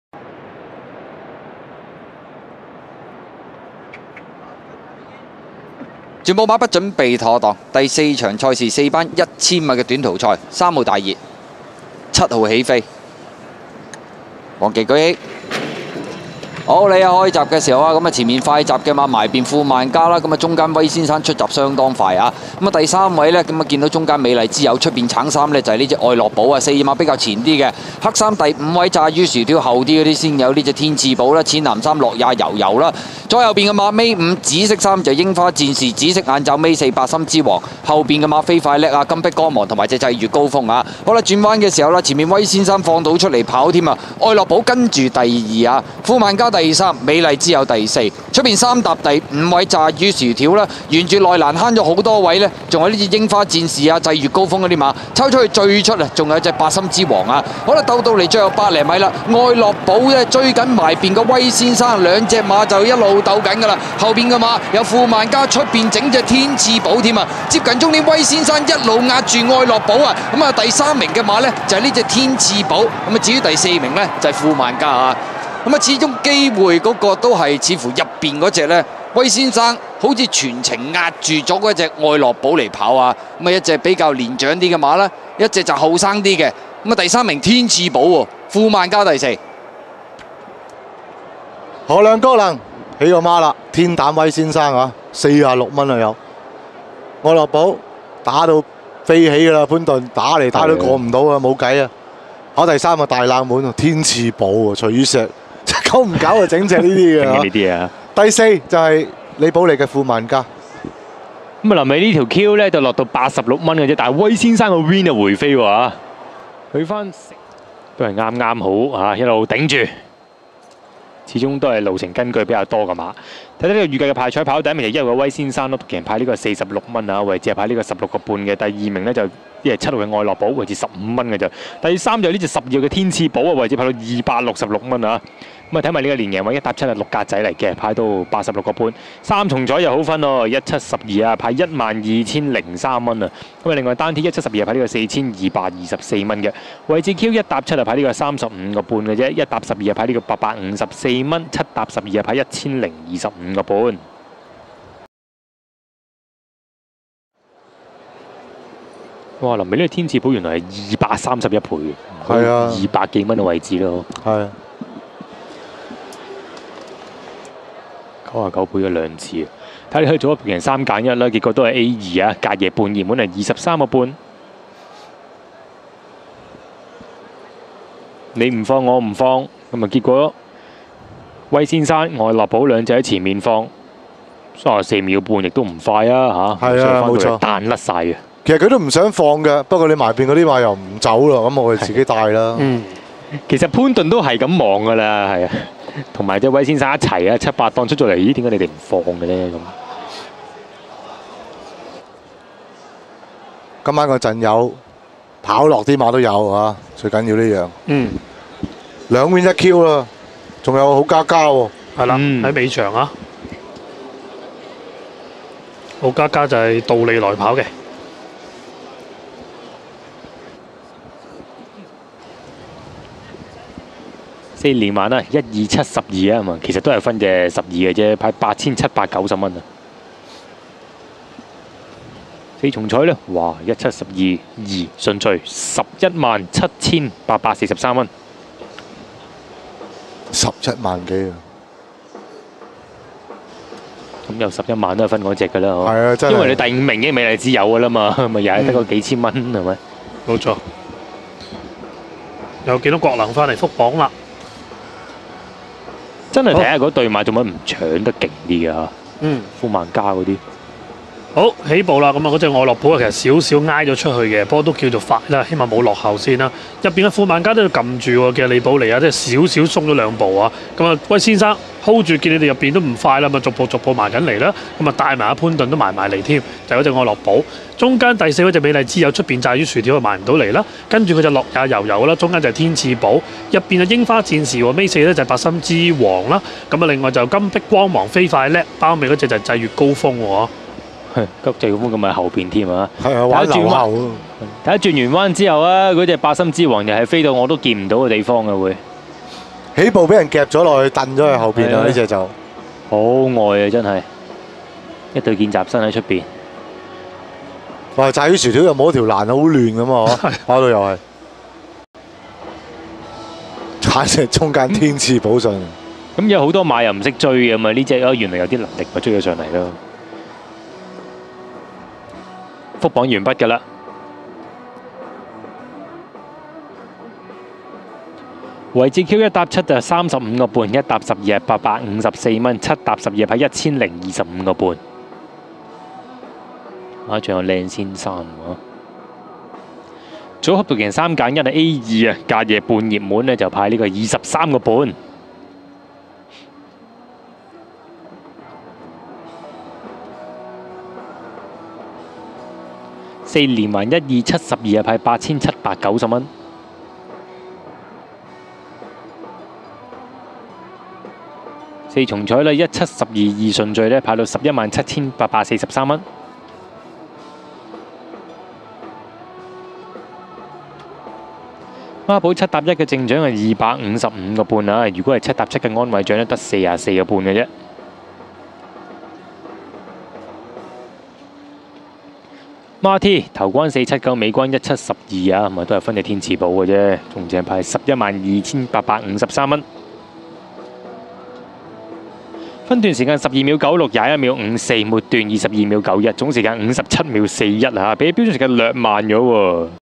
2016/17 (584) - Mr Right (威先生) - 郭能 (落班出擊再次落飛得手)
Jockey : N Callan (郭能)
Trainer : L Ho (何良)
Owner : Kenneth Shi Ho Ming (史昊洺)

2017/04/20 - Hong Kong - Happy Valley (584)